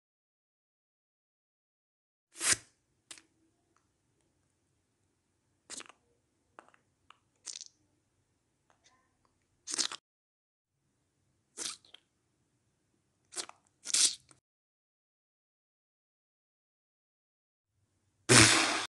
What is this in a Spider-Man mask?peter parker cant wear this mask! claymation by claykiller
Hey Guys! im clay killer and i make Stop-Motion animation.